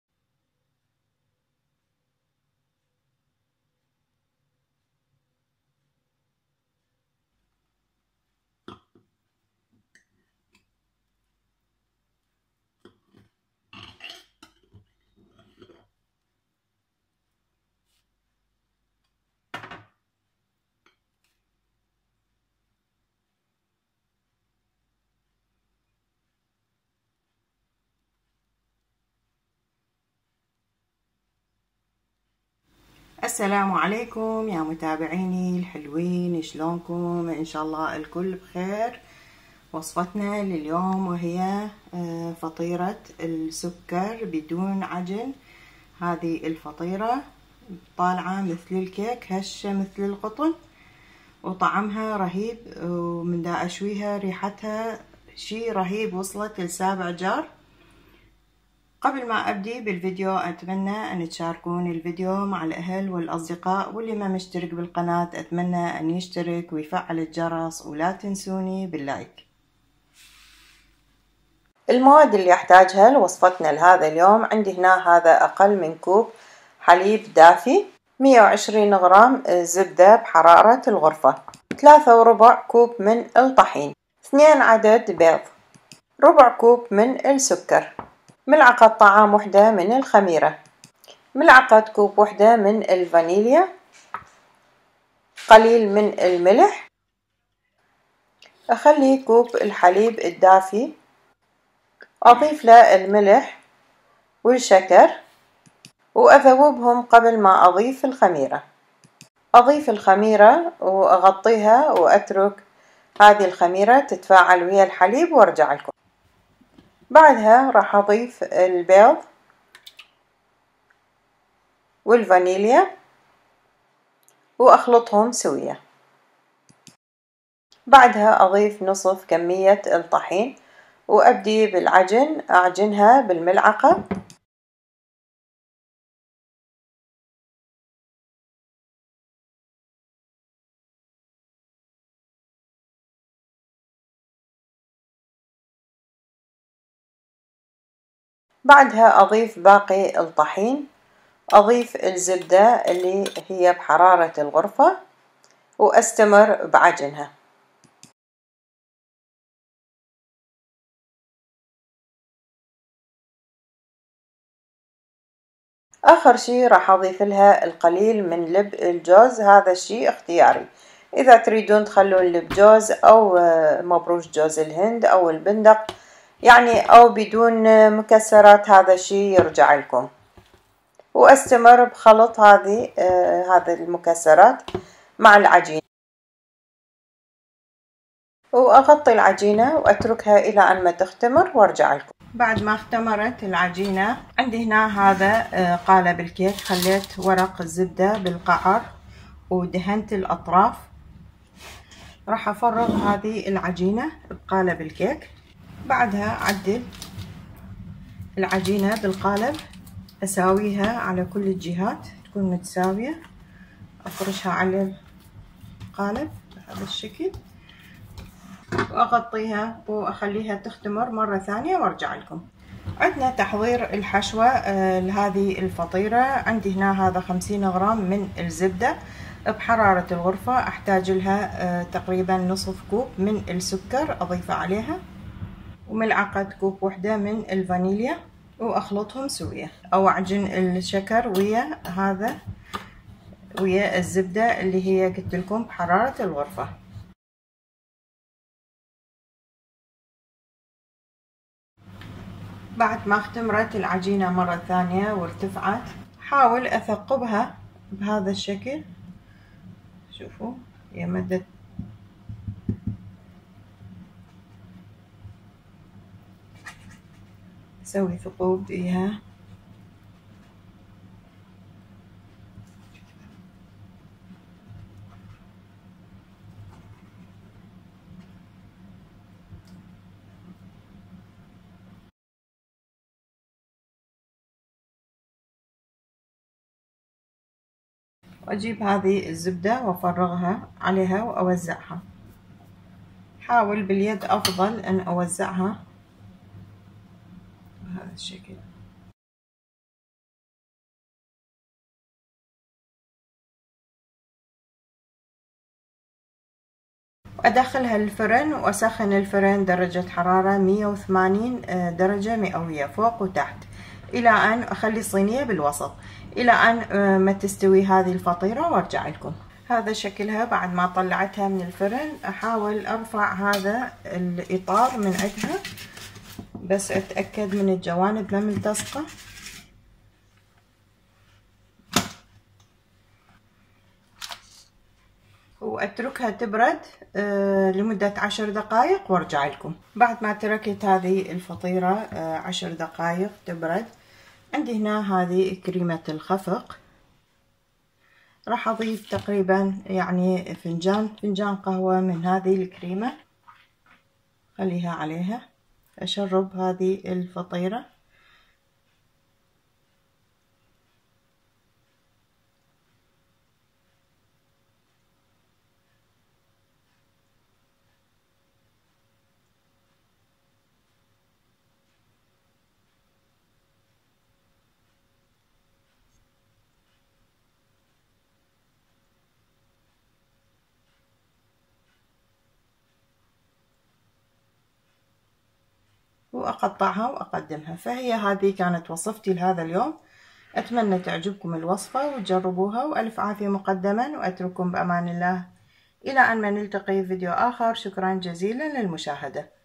بدون عجن فطيرة السكر الشهيرة خفيفة قطنية لينة ولذيذة جدا
المقادير
٢٠٠ مل حليب دافي= اقل من كوب
١/٤ كوب سكر
١ ملعقة طعام خميرة
١ ملعقة كوب فانيلا
١/٢ ملعقة صغيرة ملح
٢ بيض
١٢٠ غرام زبدة بحرارة الغرفة
٣ وربع كوب طحين
لب جوز او اي مكسرات حسب الرغبة
للحشوة
٥٠ غرام زبدة بحرارة الغرفة
١/٢  كوب سكر
١ ملعقة كوب فانيلا
١ فنجان قهوة كريمة الخفق